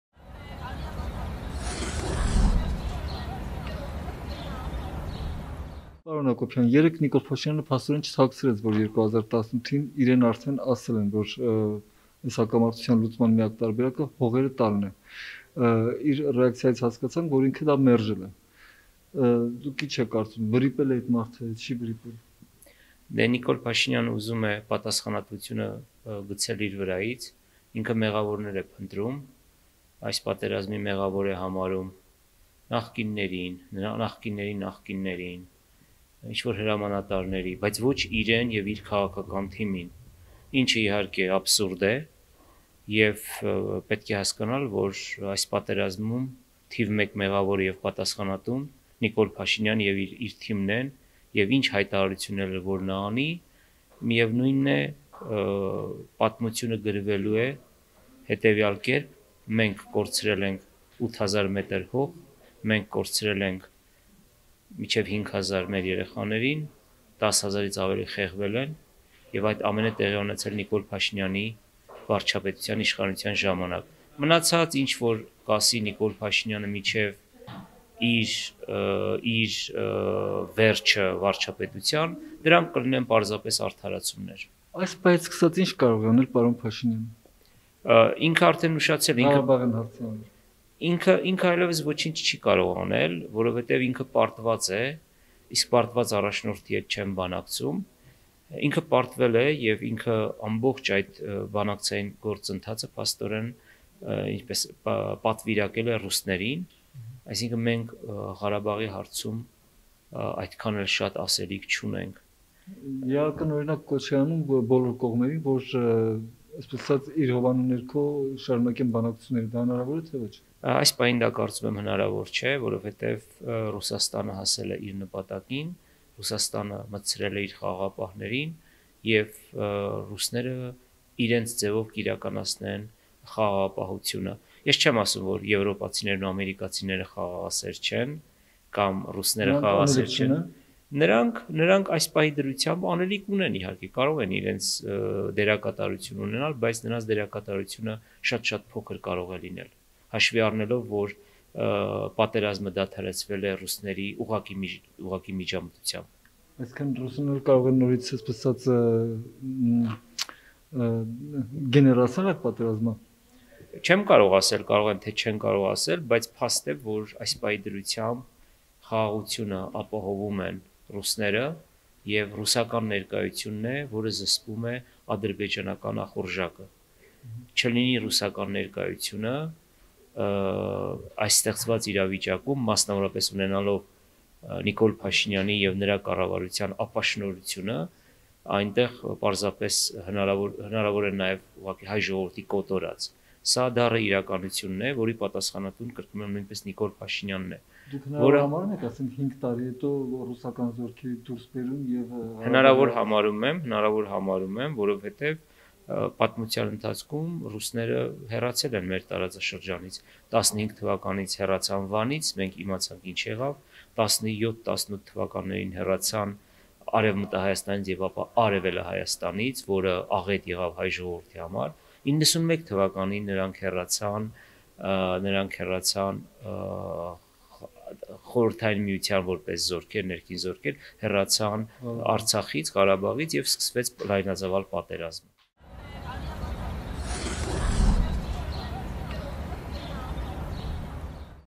Հնարավոր է 5 տարի հետո ռուսները հեռանան Արցախից․ Թաթուլ Հակոբյանի զրույցը New Armenia-ի հետ
Newarmenia. am

ԱՆԻ կենտրոնի համակարգող Թաթուլ Հակոբյանը Նիկոլ Փաշինյանին չի տեսնում Ղարաբաղի հարցի լուծման բանակցող: Հարցազրույցում նա խոսում է այն մասին, թե ովքեր են հարցի լուծման իրական պատվիրակները և այն մասին, թե արդյո՞ք ռուսները կարող են պատճառ դառնալ նոր պատերազմի։